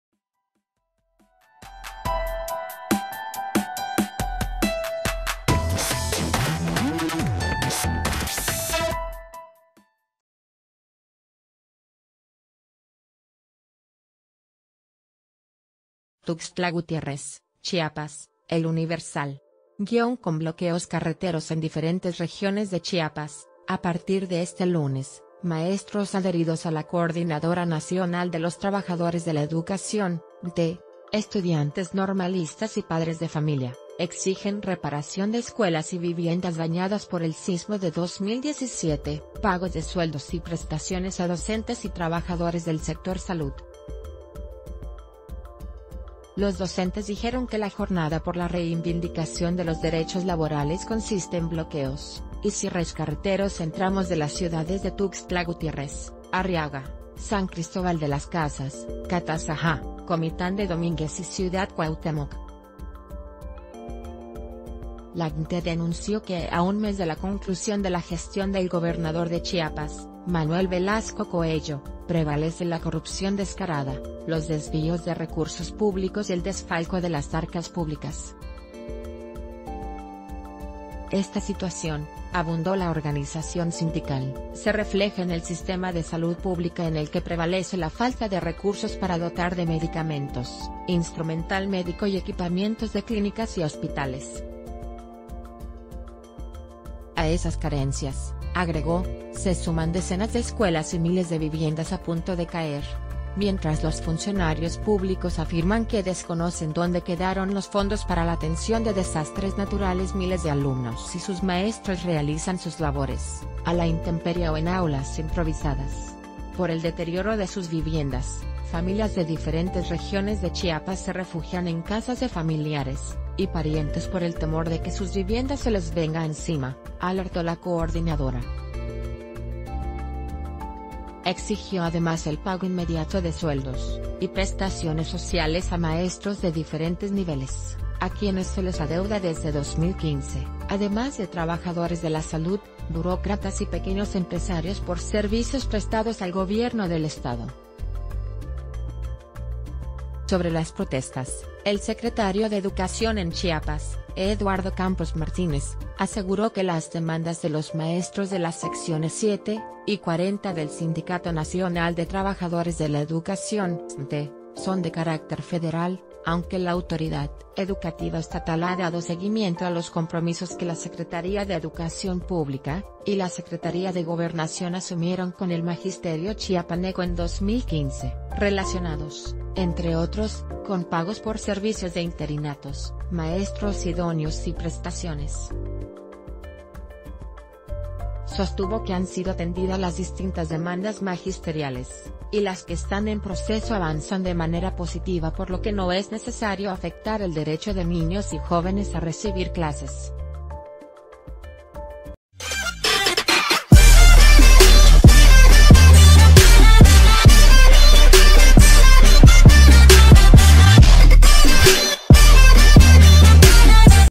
Maestros realizan bloqueos carreteros en Chiapas - El Diario de Yucatán
TUXTLA GUTIÉRREZ, Chiapas (El Universal).- Con bloqueos carreteros en diferentes regiones de Chiapas, a partir de este lunes, maestros adheridos a la Coordinadora Nacional de los Trabajadores de la Educación (CNTE) estudiantes normalistas y padres de familia, exigen reparación de escuelas y vivienda...